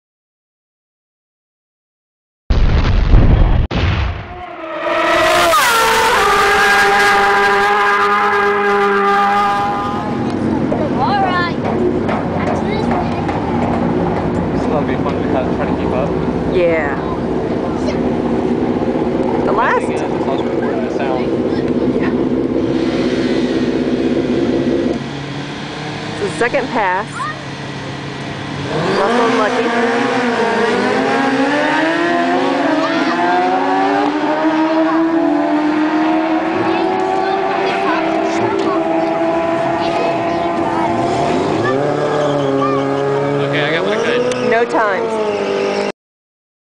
Shake-down run of track bike
MotoSpeed.us member purchased a track bike, and took the bike to the local drag strip for a few shake-down runs prior to putting the bike on a road racing course.